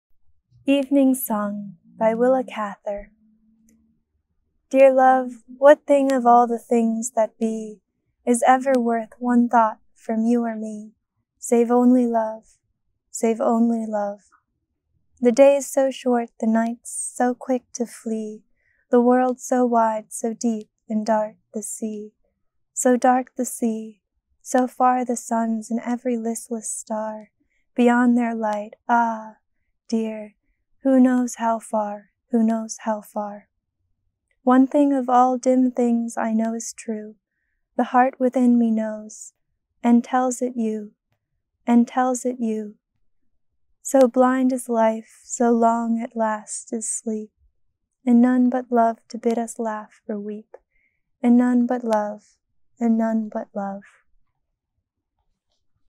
"Evening Song" ~ a poem by Willa Cather
Evening Song
by Willa Cather

Dear love, what thing of all the things that be
Is ever worth one thought from you or me,
Save only Love,
Save only Love?

The days so short, the nights so quick to flee,
The world so wide, so deep and dark the sea,
So dark the sea;

So far the suns and every listless star,
Beyond their light—Ah! dear, who knows how far,
Who knows how far?

One thing of all dim things I know is true,
The heart within me knows, and tells it you,
And tells it you.

So blind is life, so long at last is sleep,
And none but Love to bid us laugh or weep,
And none but Love,